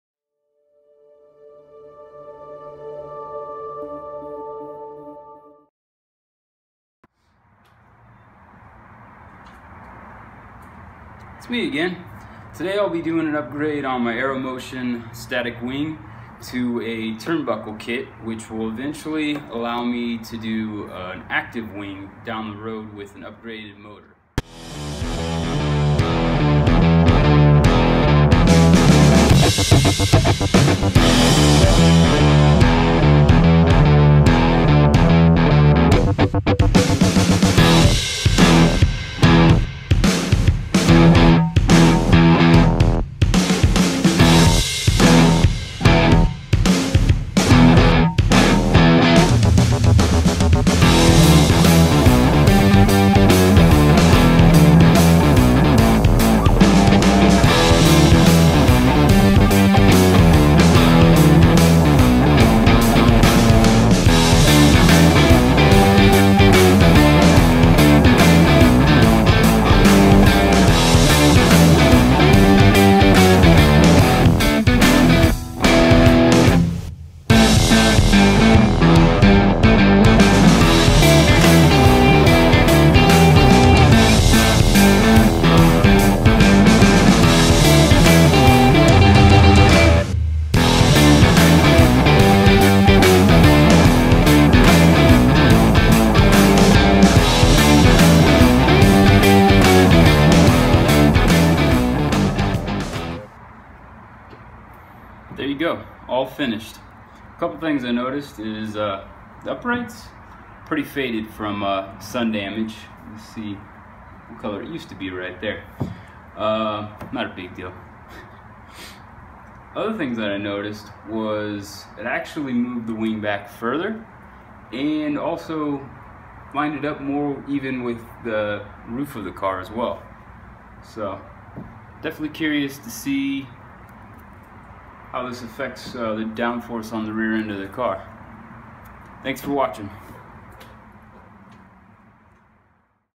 Evo 10 Aeromotion Wing Upgrade
Switched up to a turn buckle kit for my Evo X Aeromotion Wing

Follow me @daitenshituning on Instagram for more!